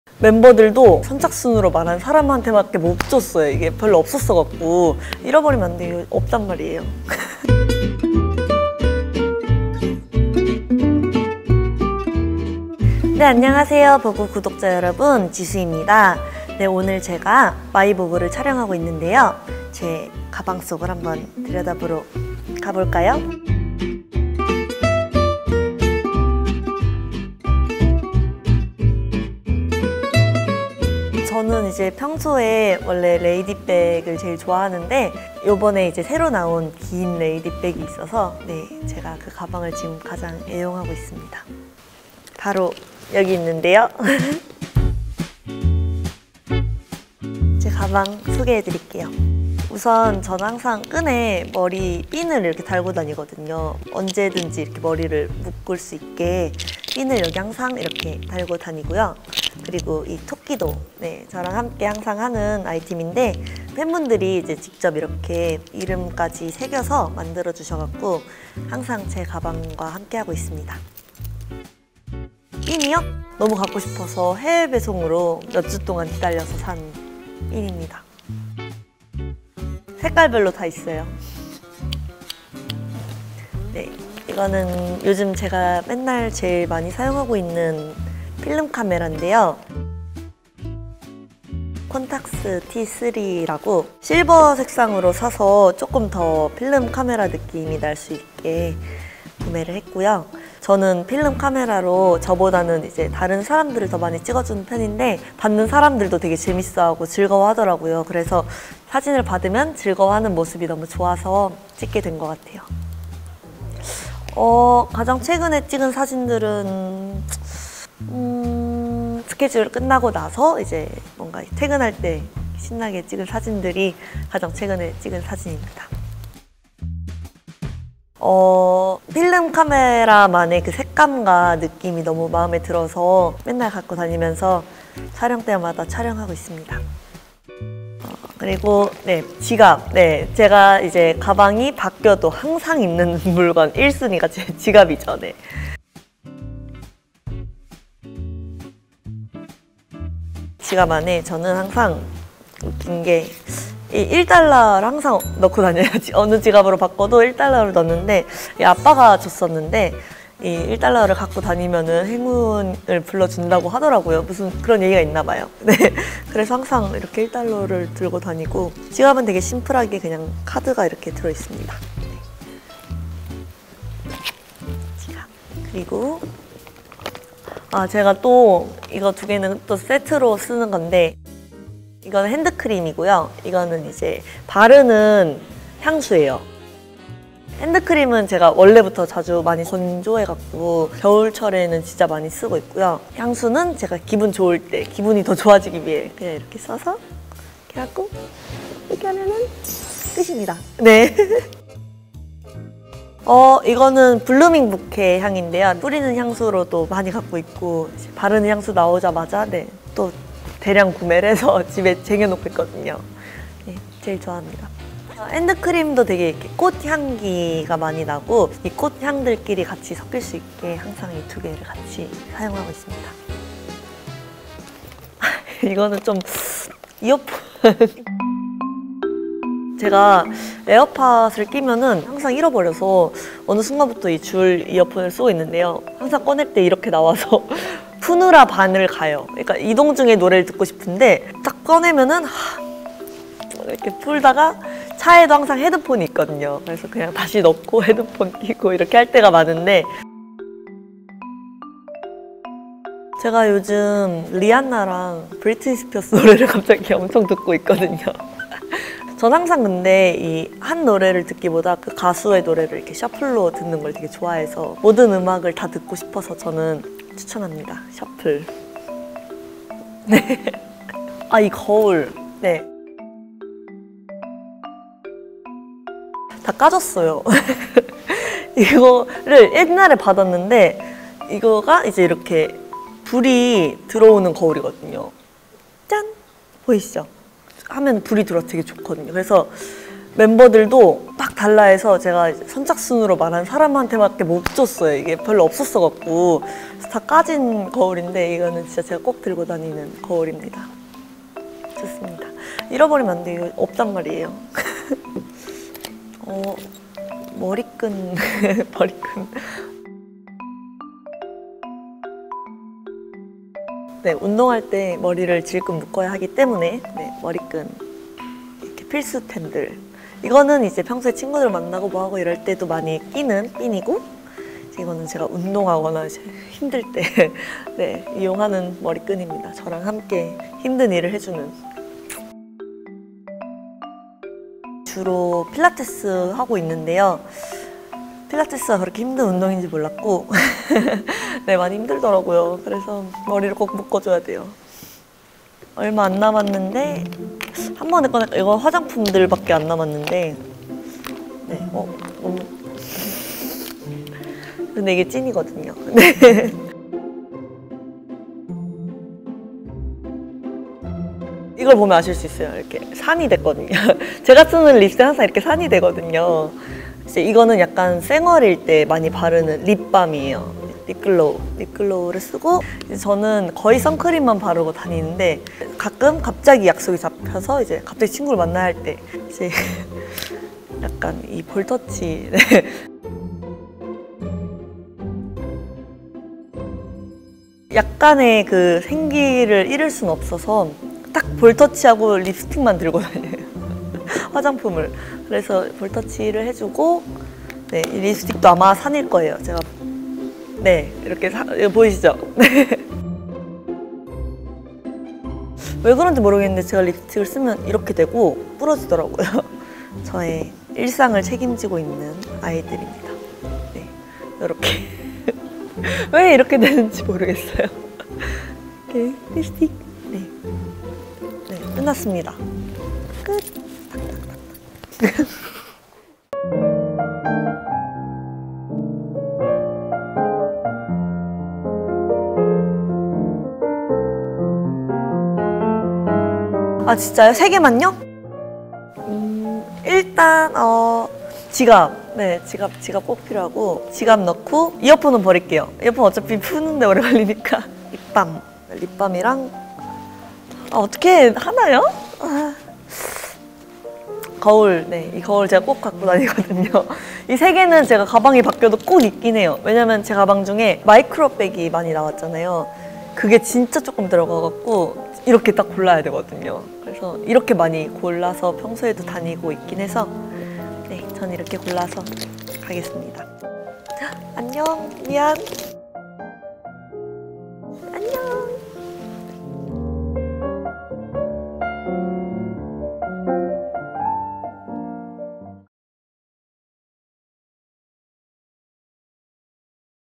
[ENG] 지수의 가방 속 최초 공개! 블핑 멤버들도 탐낸 아이템은?JISOO reveals what's in her bag for the first time! | MY VOGUE
요즘 지수가 가장 애정하는 가방 그리고 지수의 일상을 책임지는 아이템을 공개합니다. 블랙핑크 멤버들도 다 갖고 싶어했다는 ‘이것’과 미니 백을 들 때도 결코 포기할 수 없는 세 가지는? 지금 영상으로 확인하세요.

Find out JISOO's favorite bag and items that she can't live without. If you're curious about which item all members of BLACKPINK wanted, and what JISOO always carries around even when she has a mini bag on her, check it now!

VOGUE loves JISOO
지수의 모든 화보와 인터뷰, 영상은 아래 링크에서
JISOO’s full pictorial, interview and video content

에디터 | 권민지
디자인 | 장명식
촬영 | 황상준
편집 | 장소라